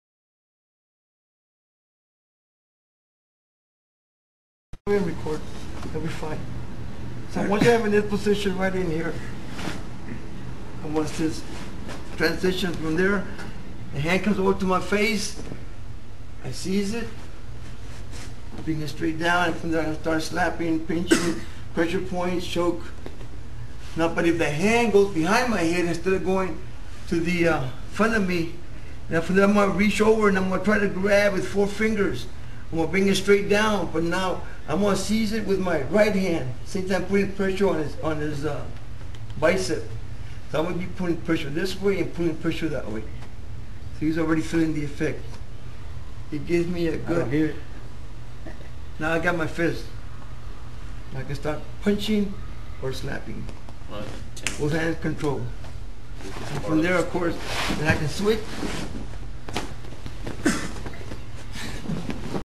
Counters While in Scarf Hold
While in Kesa Gatame, counter reach to face or reach around shoulder to head.